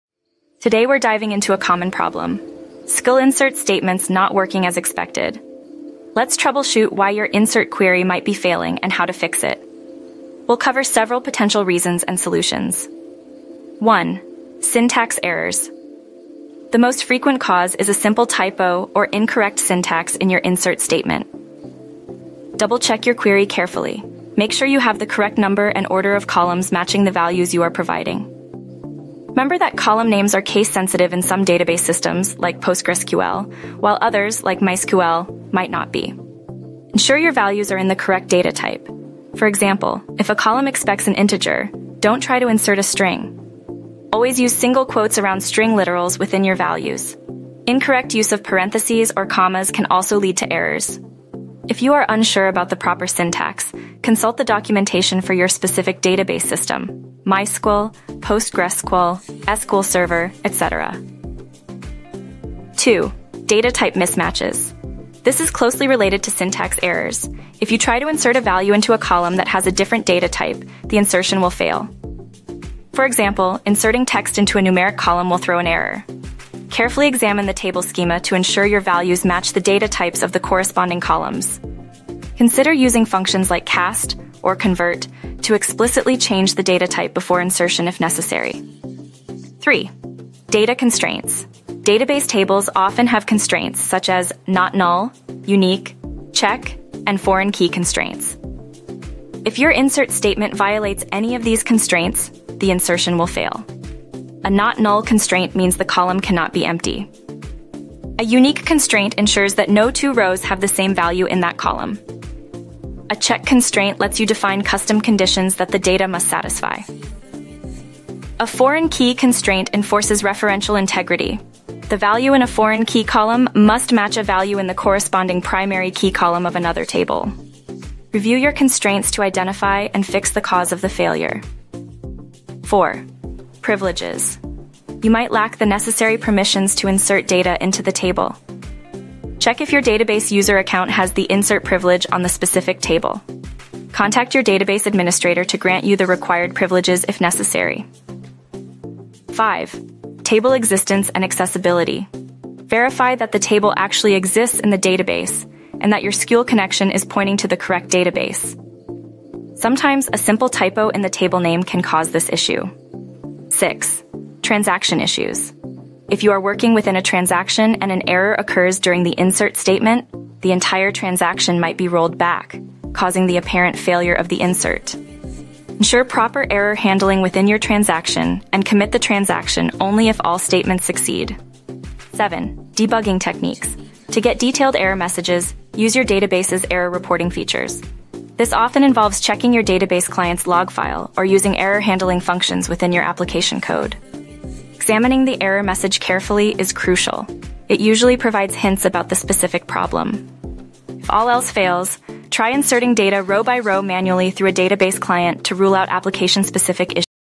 SQL: SQL Query INSERT Not Working for Inserting Values into My DB?
In this video tutorial, we will troubleshoot the common issue of SQL INSERT queries not working properly when trying to insert values into a database. We will walk through the steps to identify and resolve the problem, ensuring that your data is successfully added to your database. Watch this video to learn how to troubleshoot and fix SQL INSERT issues effectively.